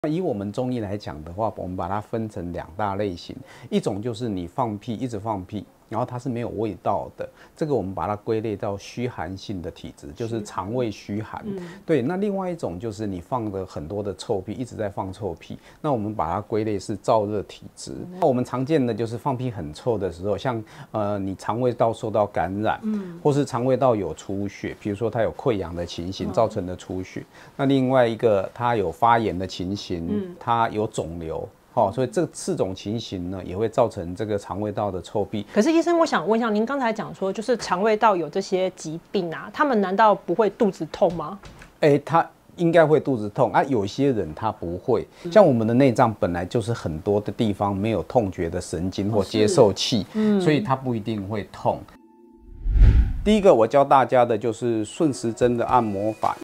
【預】臭屁？連環響屁？大腸癌前兆嗎？先從平日飲食調整起，中醫師提供茶飲跟穴道按摩，加速排除腸胃毒氣 |吳宏乾 | 健康養成記 with Amber
一天放10-20個屁都算正常，但有人就是臭屁沖天！或連環響屁！難道是大腸癌前兆？屁跟飲食與腸胃有關，醫師提供兩款茶飲跟穴道按摩，加速排除腸胃毒氣！

mainbar： 搶救臭屁！響屁！茶飲穴道雙重救援！

00:20 產屁的六種原因
03:35 屁分兩種類型
06:00 兩招改善臭屁多屁
07:29 四種病會產臭屁
09:15 屁味反應身體警訊
10:07 情緒最易影響腸胃
12:06 兩種茶飲減少多屁臭屁
13:45 早上多響屁是這個原因
Amber健康筆記：兩招穴道按摩促腸胃蠕動
15:05 順時針按摩法
15:56 胃經穴道按摩

今天專家
吳宏乾醫師
宜陞中醫診所院長
中國醫藥大學中國醫學研究所博士
著作：《特效中草藥足浴》《蛋療》《揉耳養生治病》

【hashtag】臭屁

搶救臭屁！連環響屁！先從平日飲食調整起，中醫師提供兩款茶飲跟穴道按摩，加速排除腸胃毒氣！|吳宏乾 | 健康養成記 with Amber

發佈日期：2023年 月 日
責任編輯：

新紀元4月刊【雙頭到獨裁 中共兩會改變體制】：📖現已上市🎉